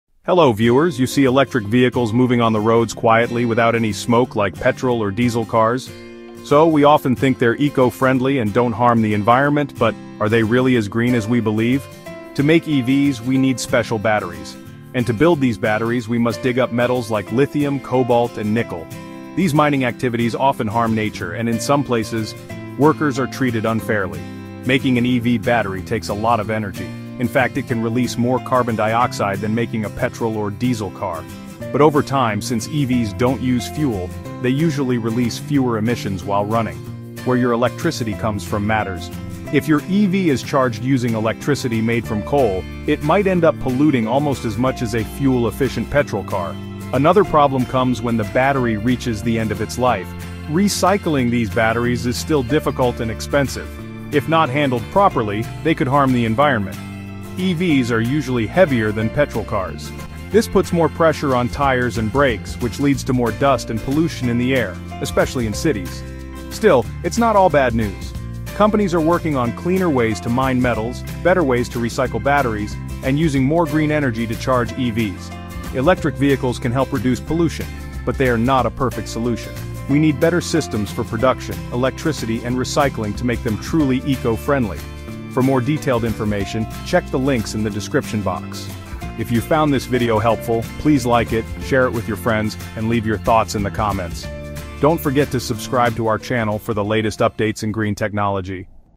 Electric Vehicles: Myths and Reality
Electric Vehicles (EVs) are often seen as the future of clean transportation. They don’t emit smoke, and they seem like the perfect eco-friendly solution. But is the reality as green as we think?

In this video, we dive into the hidden truths behind EV production, battery mining, carbon emissions, and recycling issues. From lithium mines to coal-powered charging stations, we reveal what many don’t talk about.

Yes, EVs can reduce emissions over time—but only if we address the full picture: from how they're made to how they're powered and recycled.

🛠️ Sources & Further Reading:
• NPR – EV batteries' environmental impact
• MIT Climate Portal – CO₂ emissions from battery manufacturing
• Nature – Life cycle environmental impact of EVs
• Wired – The climate cost of battery materials

1. Microplastic - An Unknown Danger
2. Understanding Optics: Chapter 1 - Reflection
3. Physics Simplified: Part 1 - Newton's Laws of Motion